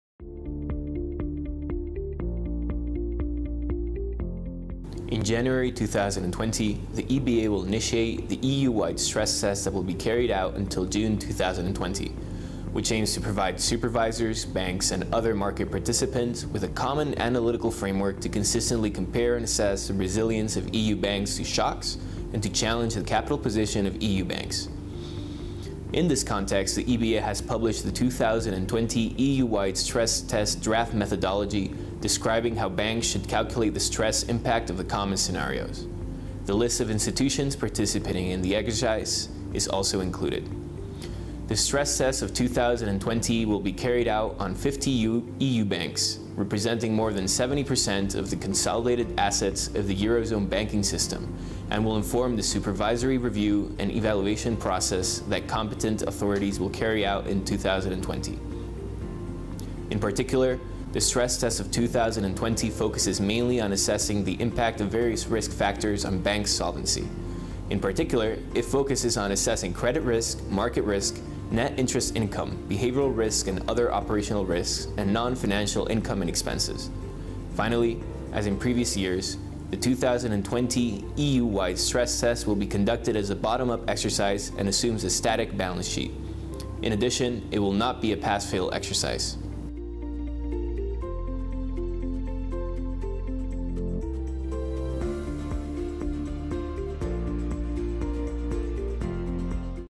2020 EU-wide stress test draft methodology (EBA)
In January 2020, the EBA will initiate the EU-wide stress test that will be carry out until June 2020, which aims to provide supervisors, banks and other market participants with a common analytical framework to consistently compare and assess the resilience of EU banks to shocks and to challenge the capital position of EU banks.

In this regard, the EBA has published the 2020 EU-wide stress test draft methodology, describing how banks should calculate the stress impact of the common scenarios. The list of institutions participating in the exercise is also included.

This exercise is designed to inform the Supervisory Review and Evaluation Process (SREP) that competent authorities (CAs) will carry out in 2020.

The Technical Note prepared by Management Solutions’ R&D department includes an analysis of the main content of this document.